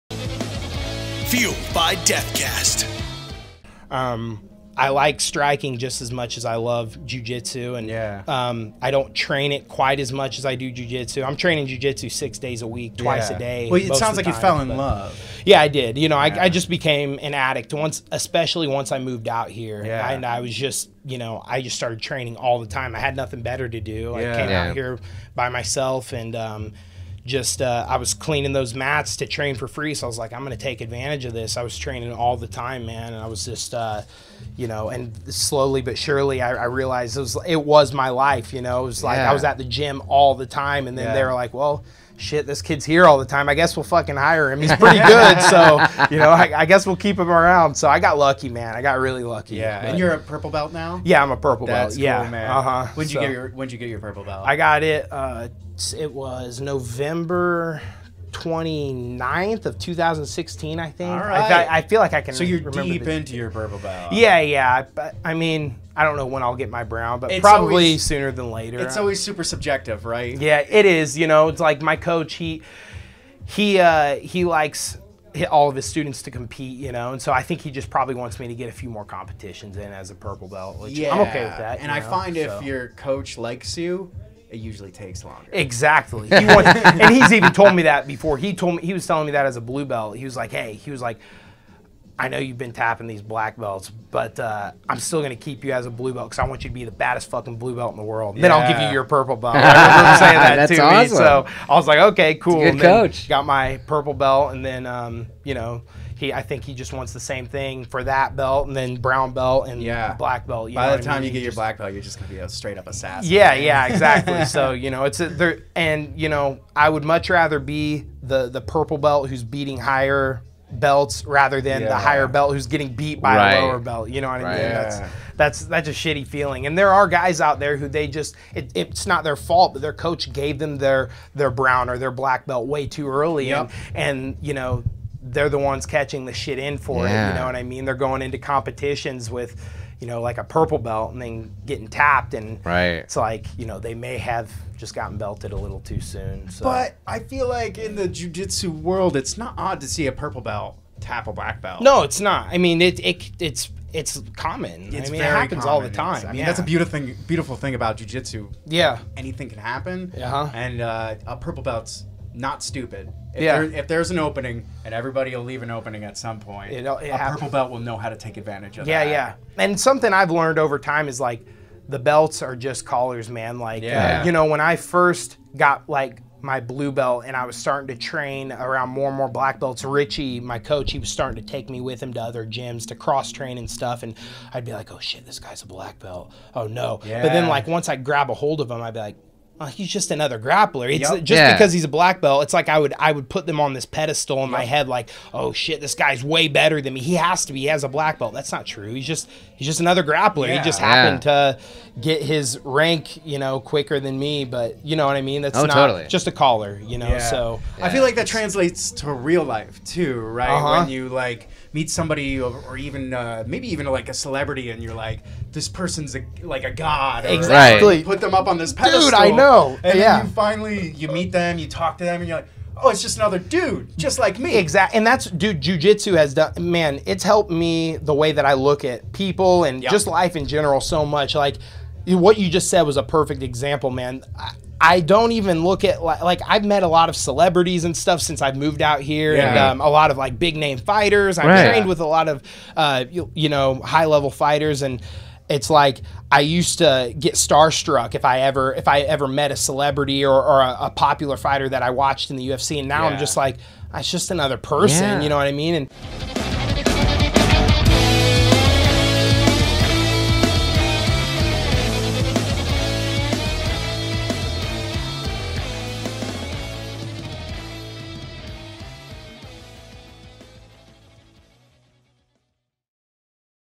Sloan Clymer on training and competing in martial arts
In this clip from episode 110, Sloan Clymer talks about training and competing in mixed martial arts. Sloan teaches and trains at 10th Planet and went from being a kid cleaning the mats to a devastating fighter and mentor.

Check out the full episode with transcript